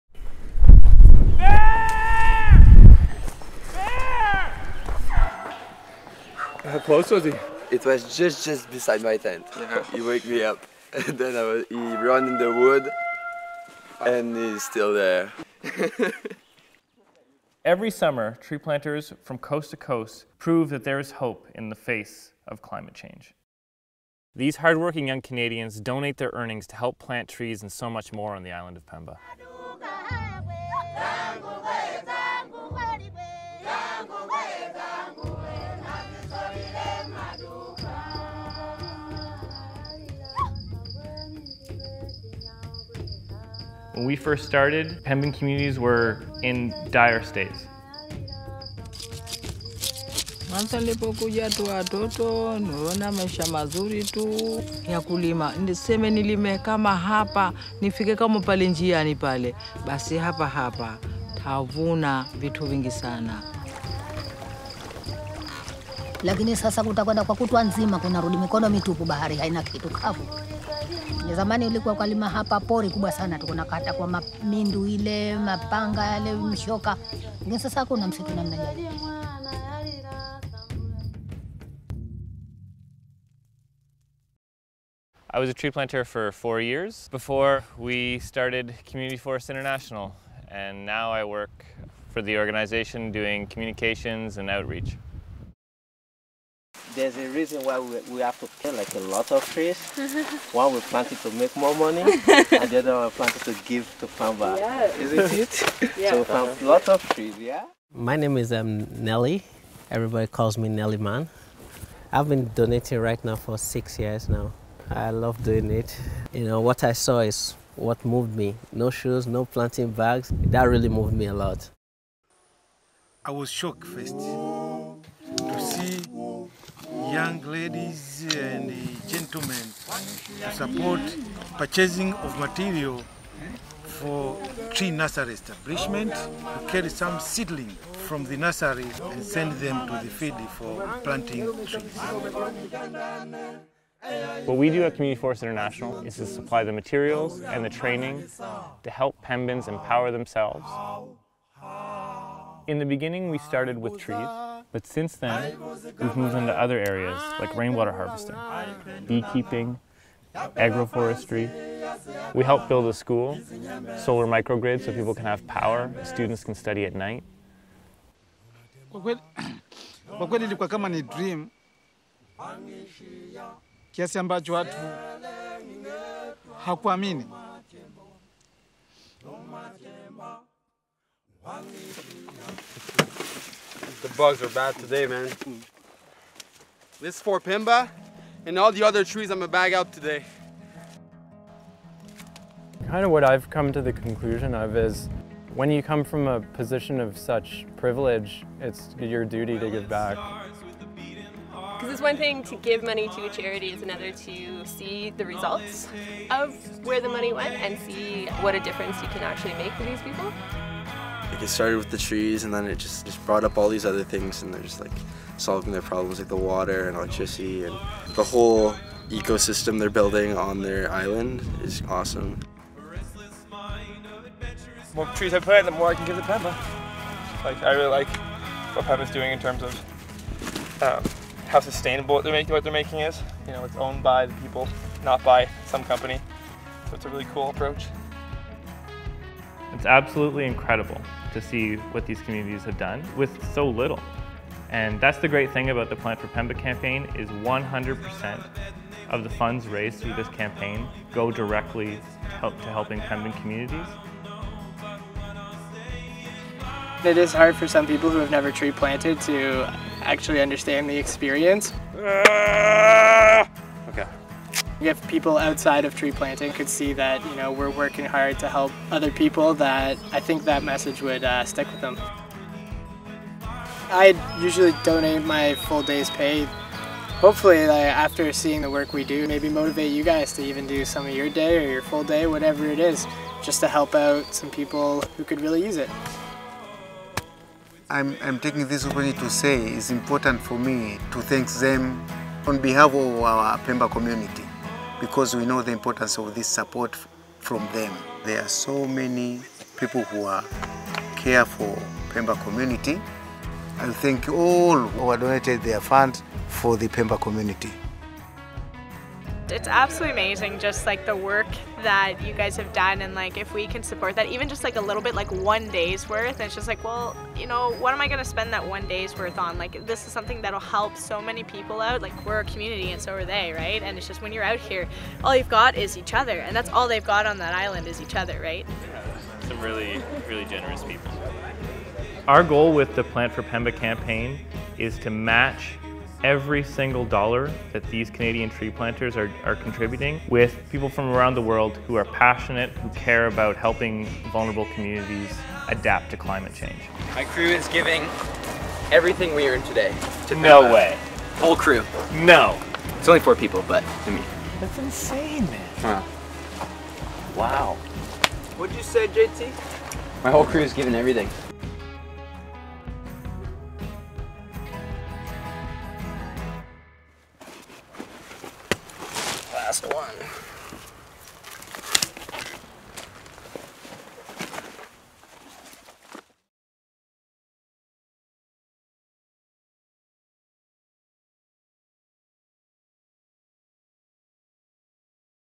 Plant for Pemba 2017
Every spring, Canadian tree-planters prove that there is hope in the face of climate change.

Over the last 9 years these incredible individuals have donated over $150,000 of their hard-earned wages to communities on the threatened island of Pemba, Tanzania. Together, they have empowered Pembans to plant over 1.75 million trees for food, shelter, and spice - helping vulnerable people increase their income while restoring their environment at the same time.

Thanks to our partners Brinkman and Associates Reforestation 100% of the proceeds donated to the annual Plant for Pemba campaign go directly to support Pemban communities.